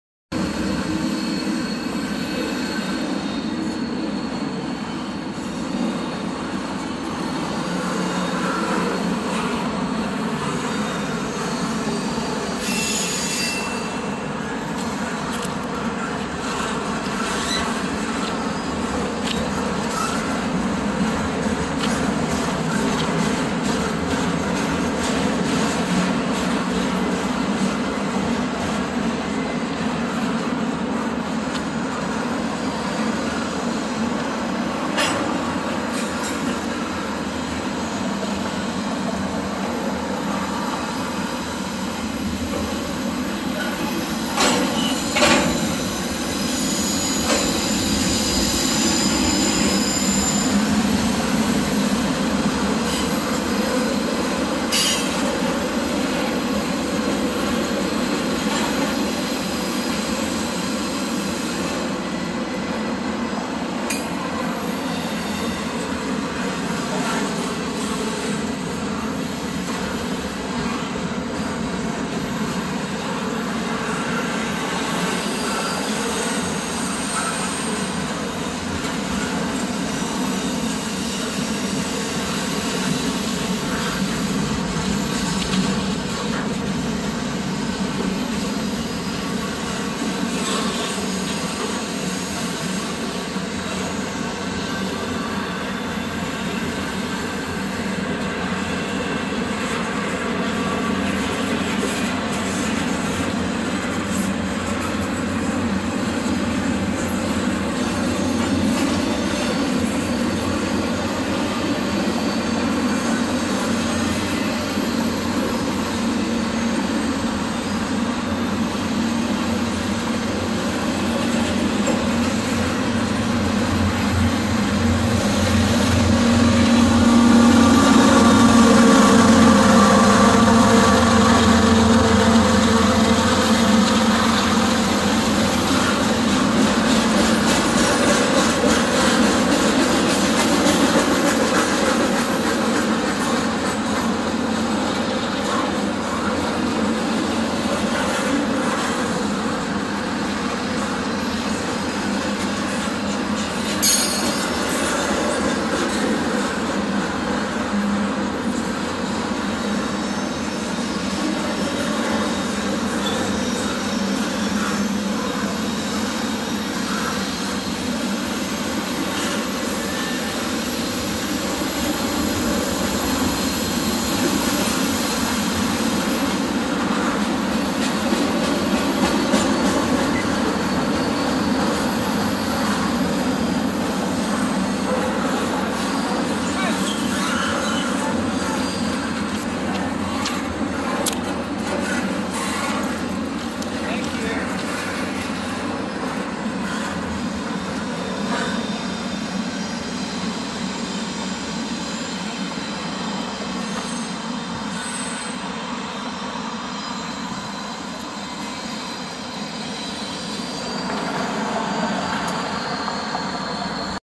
CSX double stack with DPU rolling through Cumberland,MD.
CSX double stack with a DPU rolling through Cumberland, MD. RIP Stobe the Hobo.